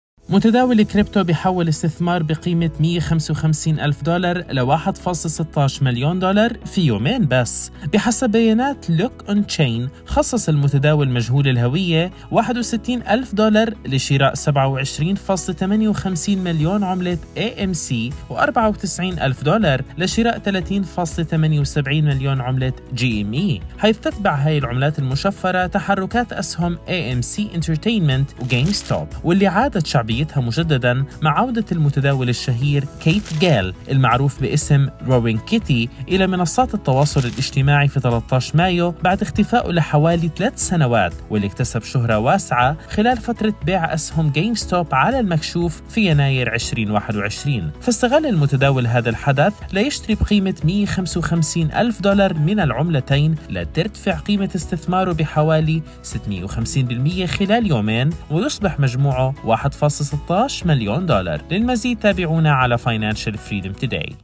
مليون دولار بالكريبتو بيومين
متداول كريبتو يستغل عودة Roaring Kitty وانتعاش GameStop ليحقق أرباحًا طائلة!